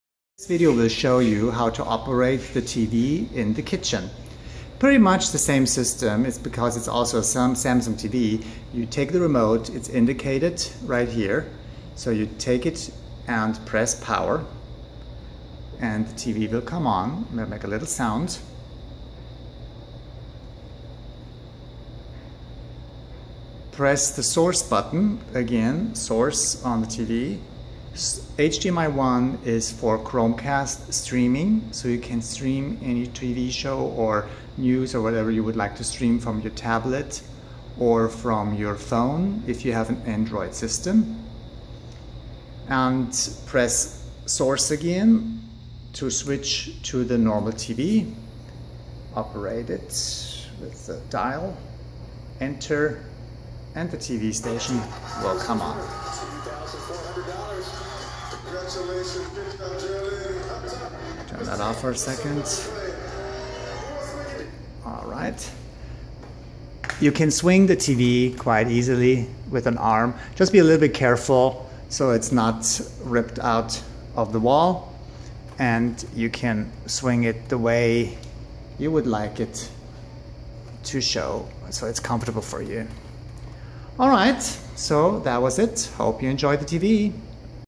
The Grand 3534 Miami-kitchen TV instructions
How to operate the kitchen TV